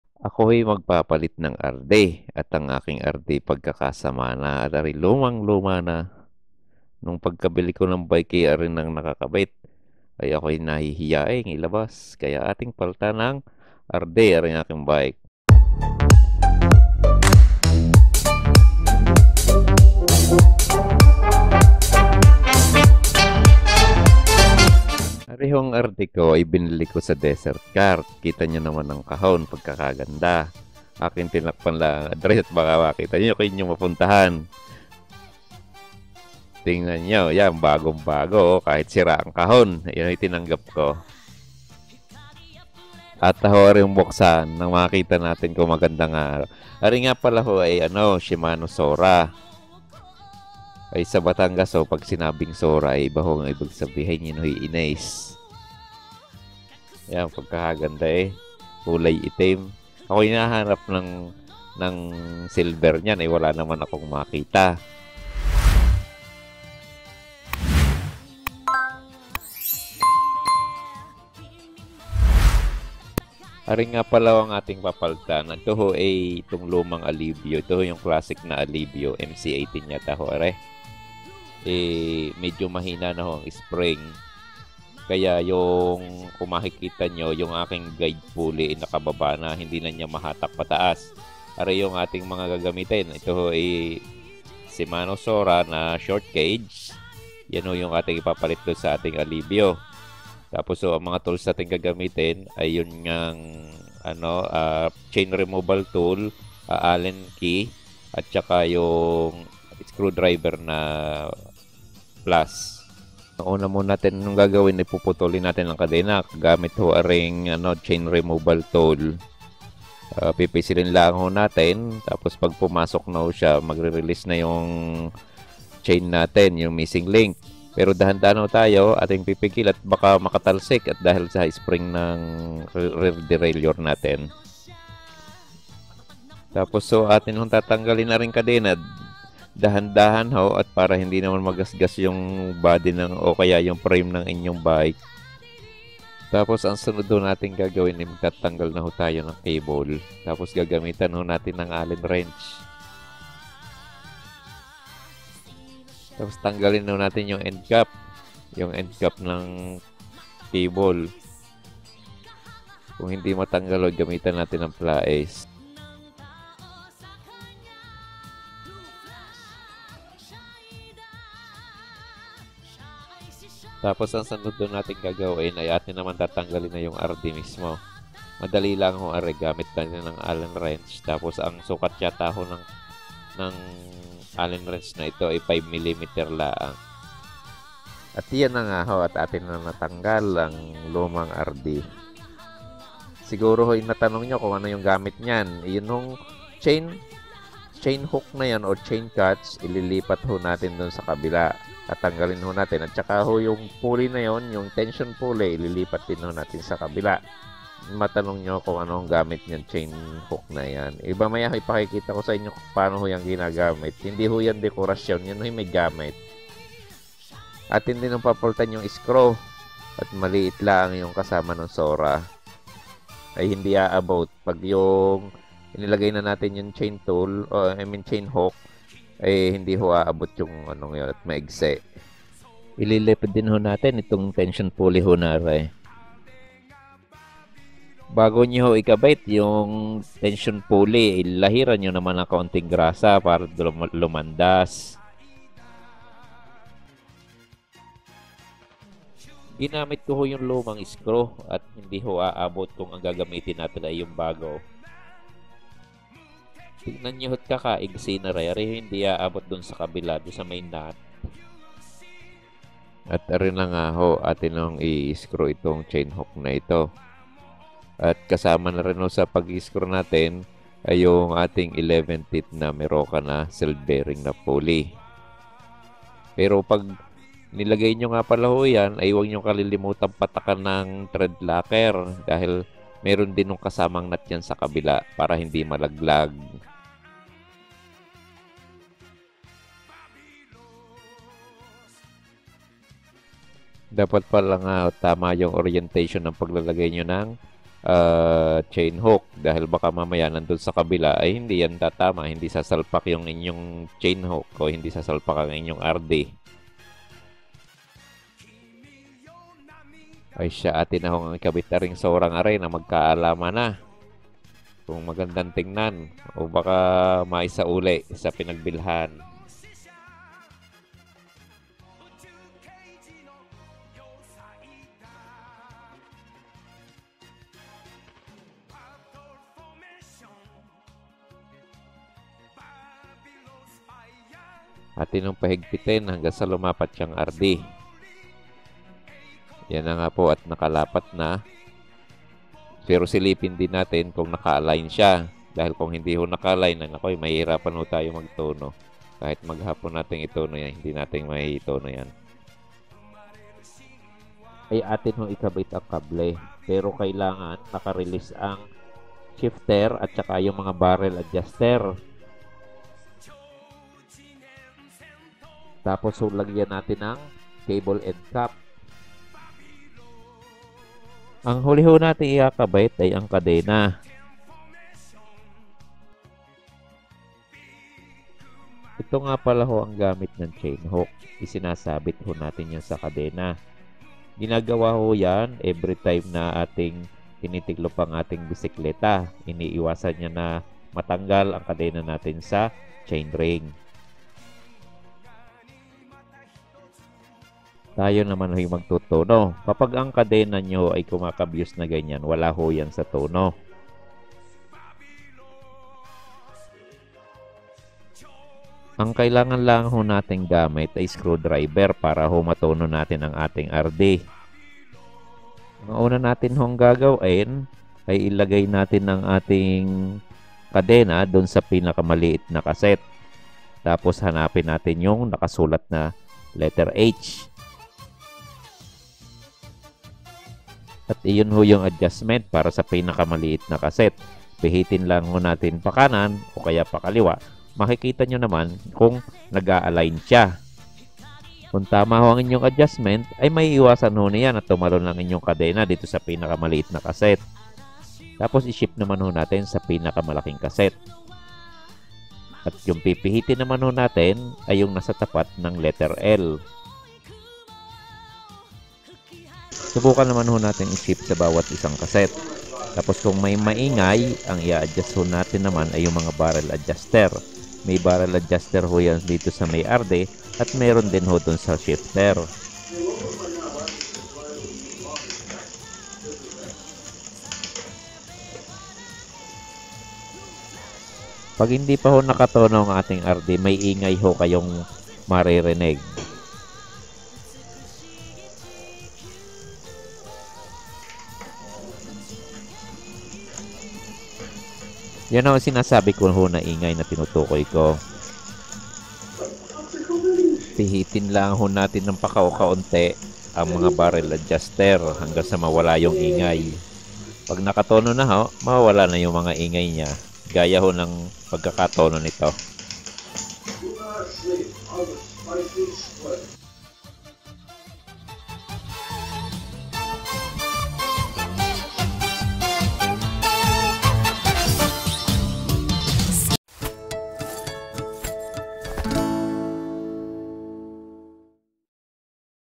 Paano magpalit at magtono ng RD (Rear Derailleur)? Rear Derailleur Upgrade | En_Gene_er
Paano magpalit at magtono ng RD (Rear Derailleur)? Rear Derailleur Upgrade

Ang video na ito ay layong magturo ng paraan ng pagpapalit at totono ng rear derailleur.